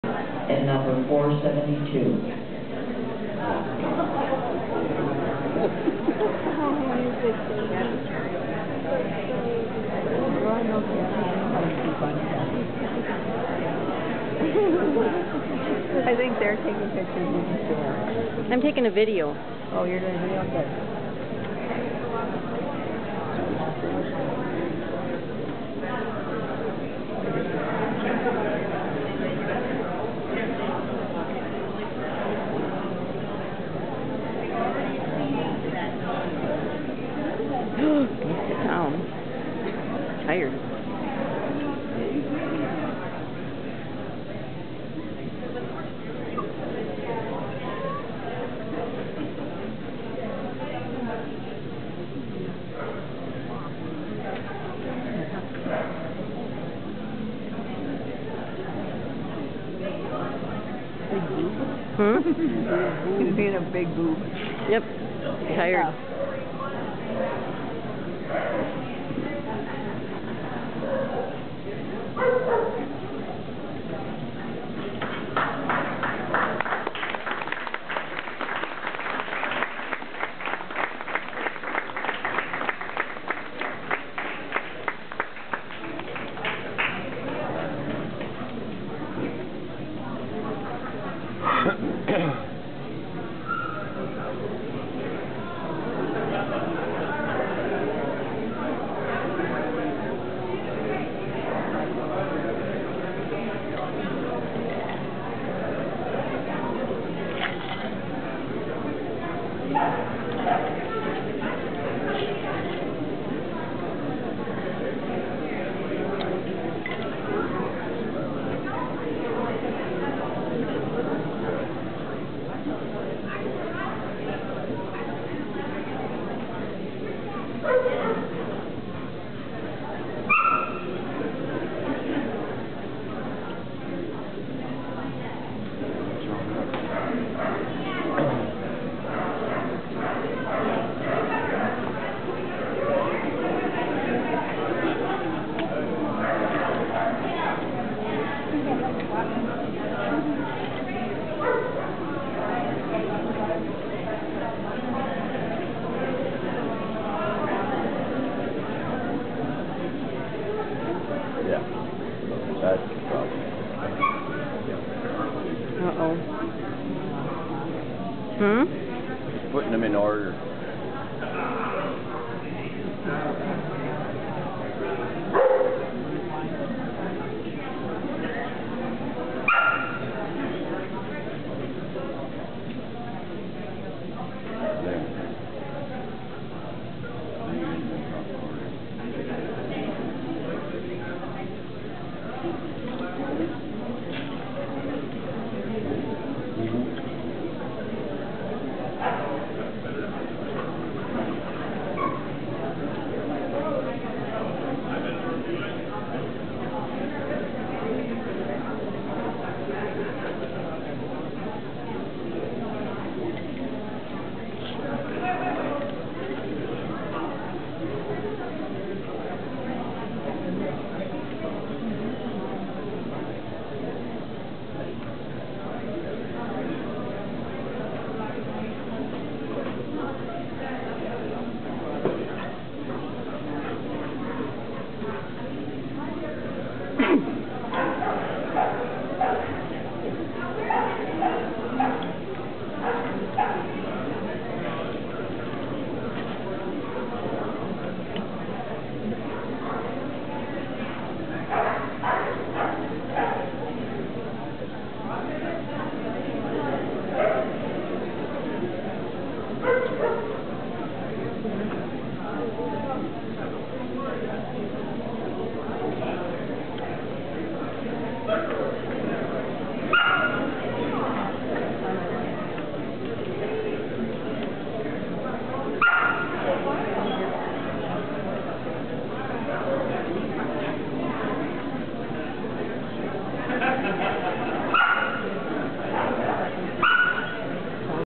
Silverwood/National Specialty 2009 White Puppy Sweeps Part 1
Emmett's first big show.  The Silverwood/BTCA National Specialty 2009 in San Jose California.